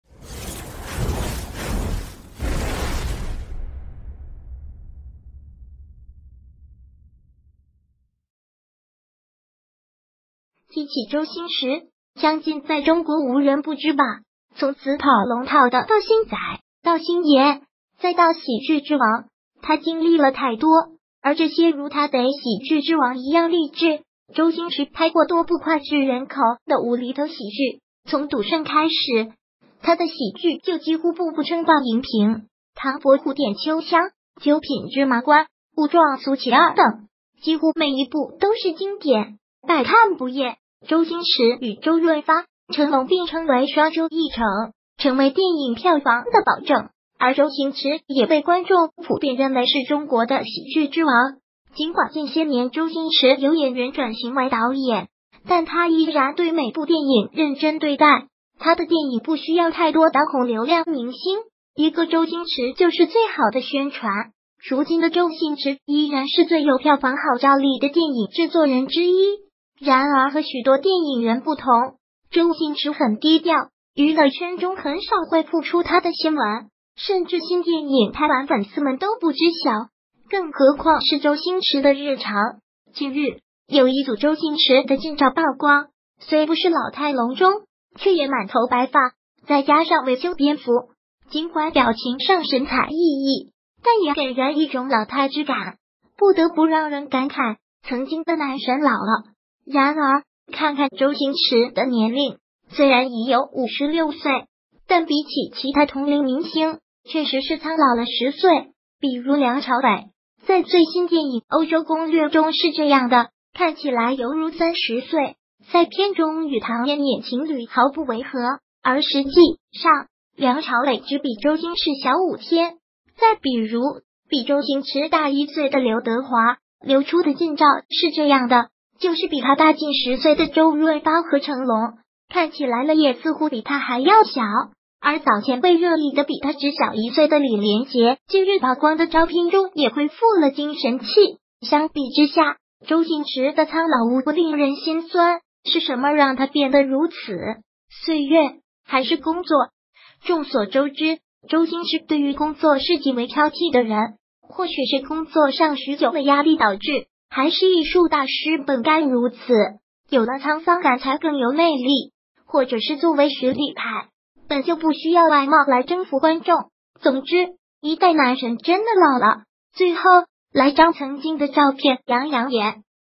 周星驰近照曝光，一代男神真的老了
提起周星驰，相近在中国无人不知吧。从“死跑龙套的”到“星仔”，到“星爷”，再到“喜剧之王”，他经历了太多。而这些如他的《喜剧之王》一样励志。周星驰拍过多部脍炙人口的无厘头喜剧，从《赌圣》开始，他的喜剧就几乎部部称霸荧屏。《唐伯虎点秋香》《九品芝麻官》《武状元苏乞儿》等，几乎每一部都是经典，百看不厌。周星驰与周润发、成龙并称为“双周一成”，成为电影票房的保证，而周星驰也被观众普遍认为是中国的“喜剧之王”。尽管近些年周星驰由演员转型为导演，但他依然对每部电影认真对待。他的电影不需要太多当红流量明星，一个“周星驰”就是最好的宣传。如今的周星驰依然是最有票房号召力的电影制作人之一。然而和许多电影人不同，周星驰很低调。娱乐圈中很少会曝出他的新闻，甚至新电影拍完粉丝们都不知晓。更何况是周星驰的日常。近日，有一组周星驰的近照曝光。虽不是老态龙钟，却也满头白发。再加上未修边幅，尽管表情上神采奕奕，但也给人一种老态之感。不得不让人感慨：曾经的男神老了！然而，看看周星驰的年龄，虽然已有56岁，但比起其他同龄明星确实是苍老了“十岁”。比如梁朝伟，在最新电影《欧洲攻略》中是这样的：看起来犹如30岁，在片中与唐